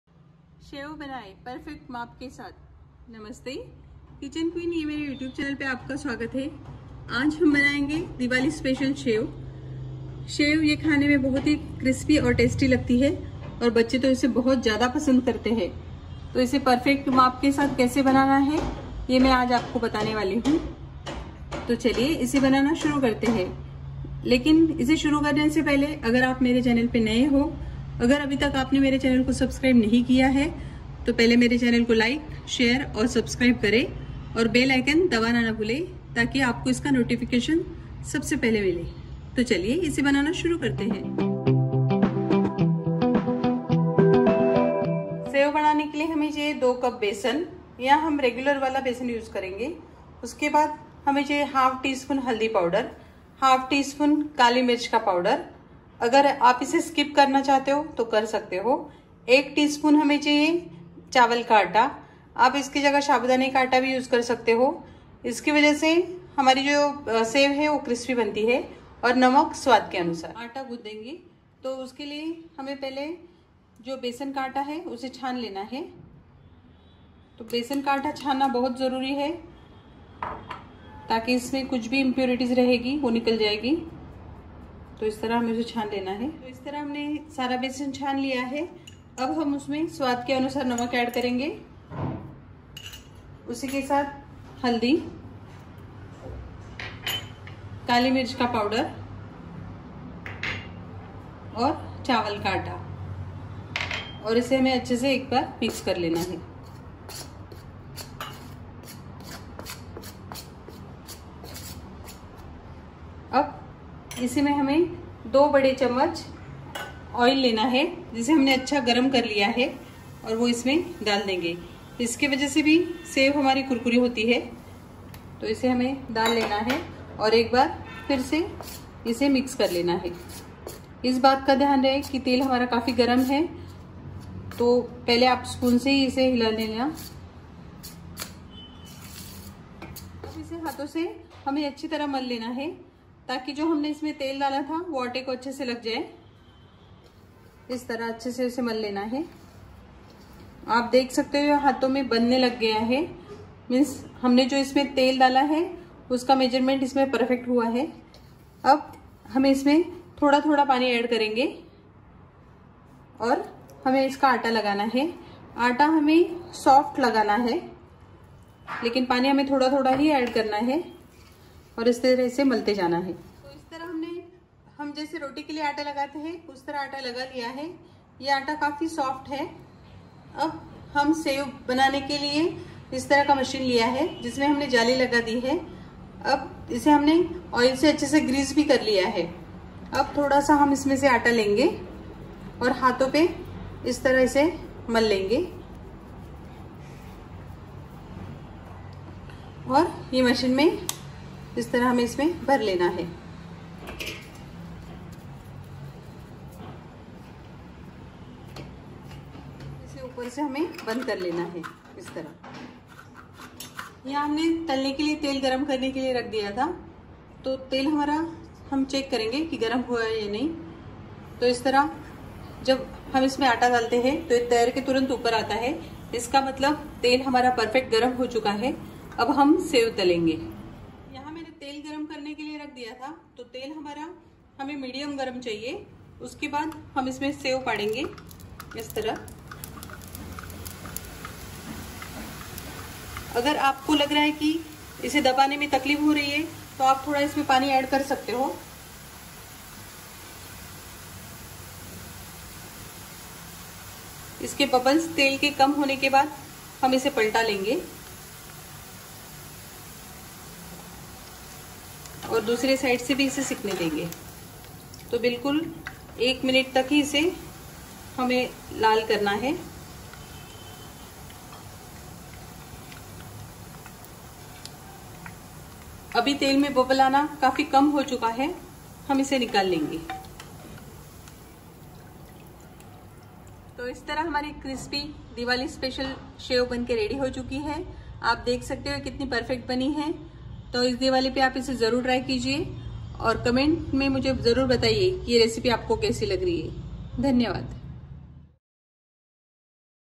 कुरकुरित शेव | Diwali Special Shev Recipe By Kitchen Queen | How to Make Shev
How to make Diwali Special Shev Recipe At Howe By Kitchen Queen